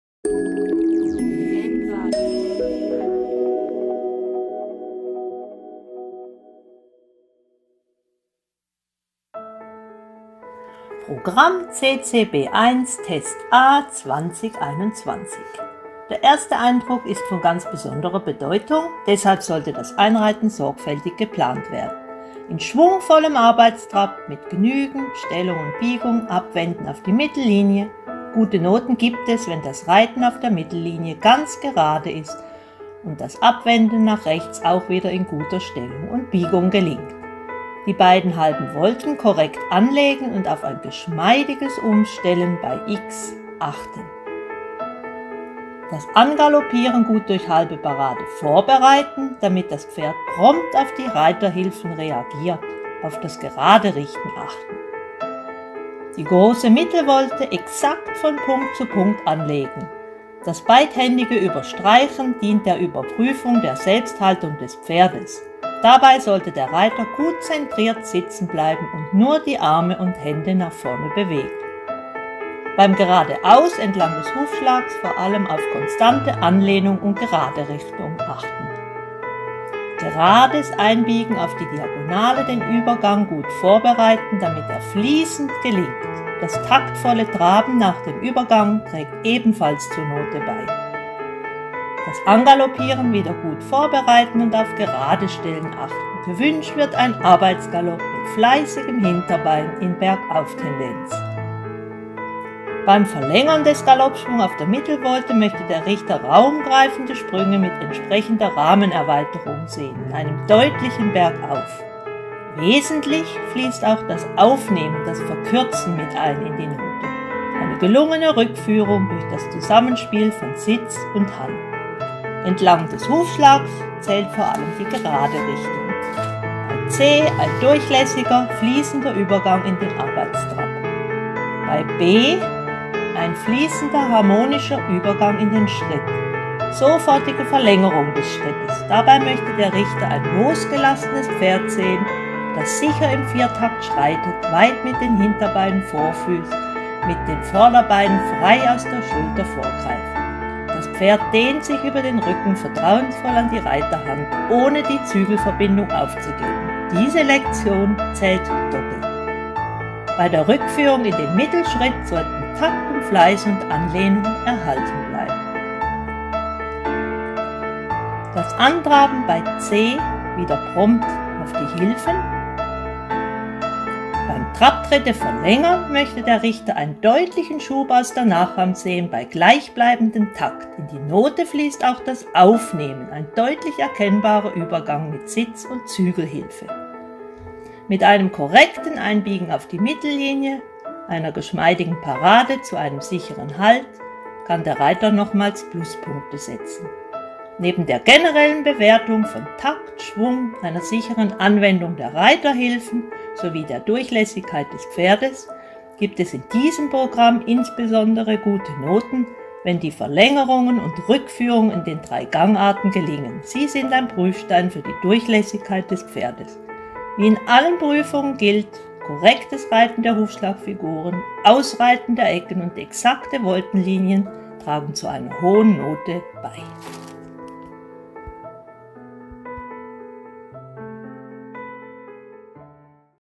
Dressurprogramm 2021 CCB1A Deutsch
CC Dressurprogramm für die Concours Complet Prüfungen in der Schweiz. Dieses Video zeigt das CCB1 Test A Dressurprogramm, welches für Basisprüfungen in der Vielseitigkeit verwendet wird.